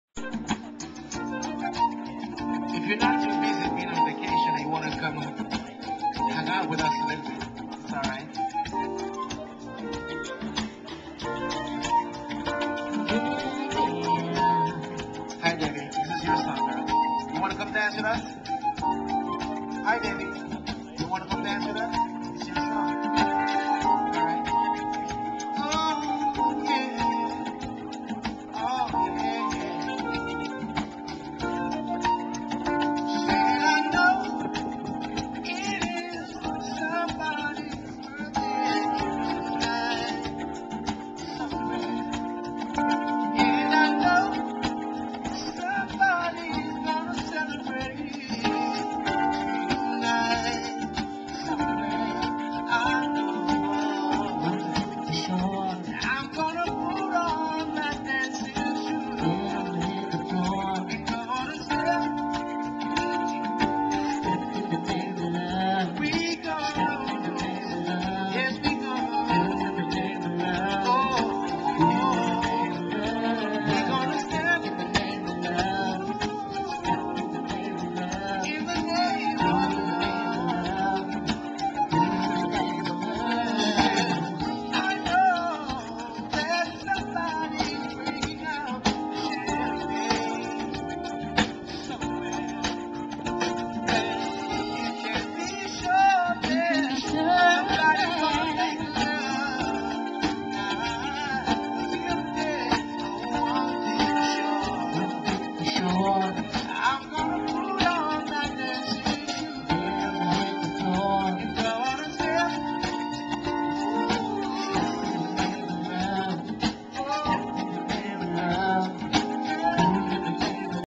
Jamaican Band "Pace"
Pace performing at Secrets Resort in Montego Bay, Jamaica. They Rocked the house! Recorded on October 14, 2010 using a Flip Video camcorder.